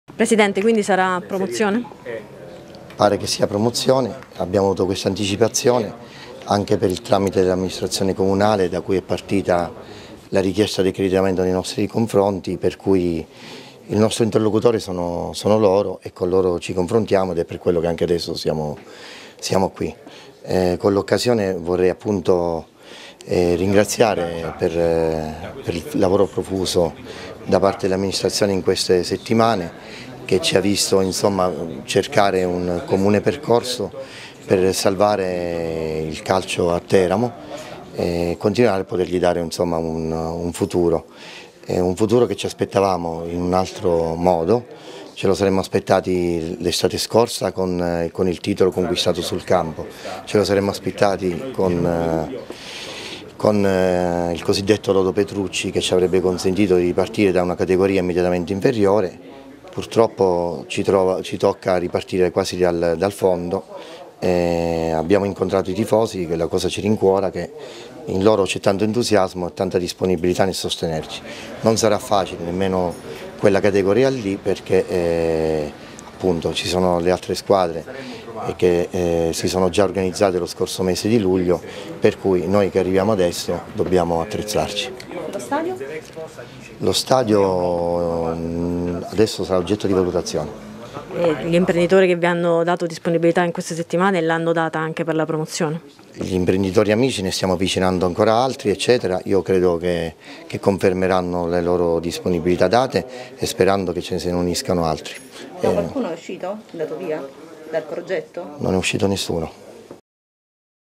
Teramo. Rastelli: “Promozione non semplice, valutazioni sul Bonolis”
Il presidente della SSD Città di Teramo, Eddy Rastelli, parla ai microfoni della ripartenza del calcio biancorosso dal campionato di Promozione con un pizzico di rammarico perché, fino alla fine, ha sperato in una categoria diversa. Per quanto riguarda il “Bonolis”, Rastelli conferma che sarà oggetto di valutazione